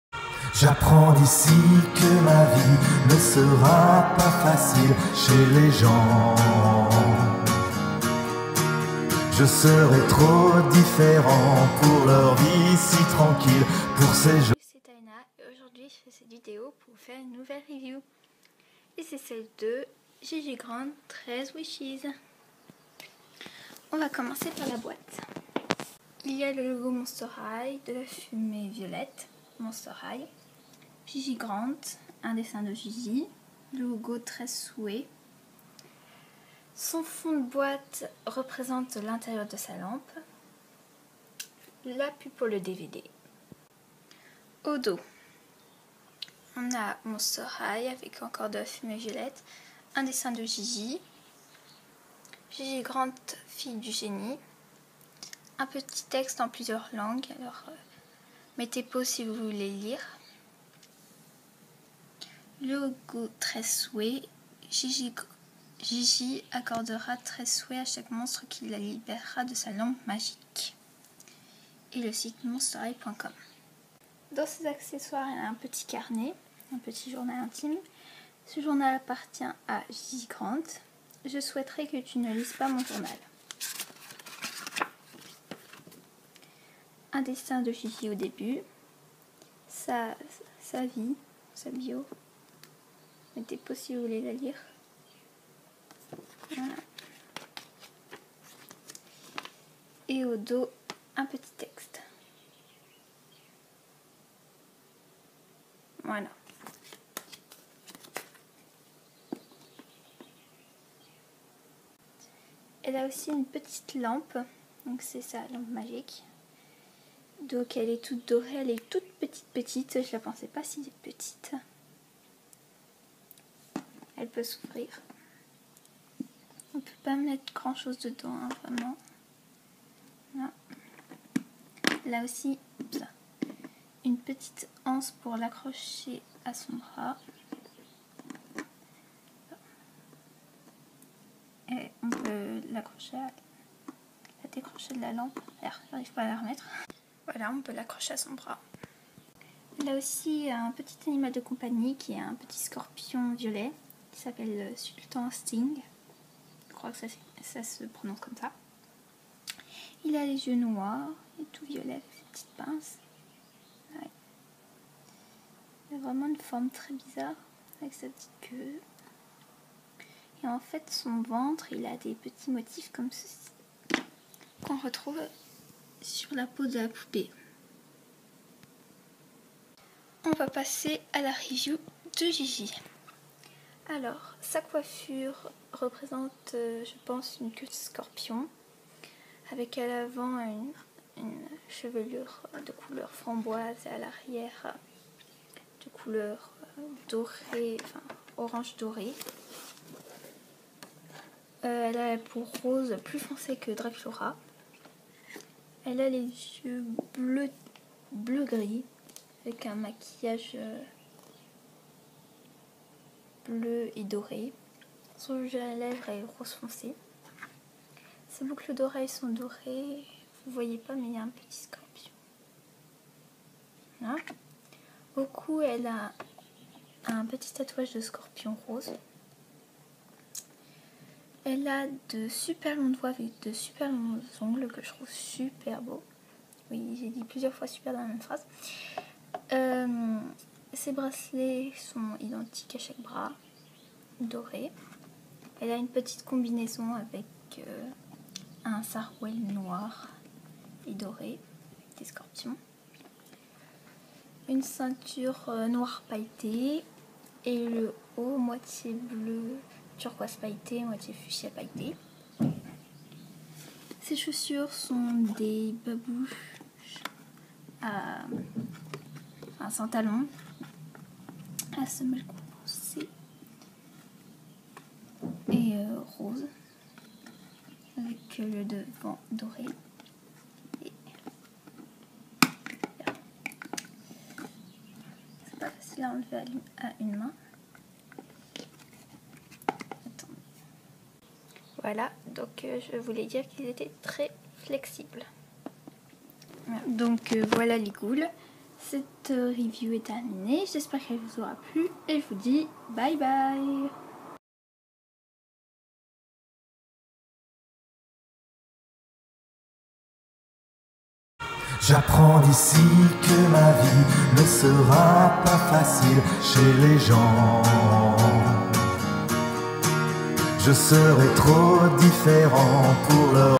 ❤ REVIEW : Monster High : Gigi Grant : 13 Wishes ❤
Aujourd'hui je fais cette vidéo pour vous présenter ma nouvelle poupée Monster High : Gigi Grant 13 Wishes!!!
INTERDIT DE COPIER OU DE METTRE UN(DES) COMMENTAIRE(S) OFFENSANT(S)!!!
Merci d'avoir lu cette description.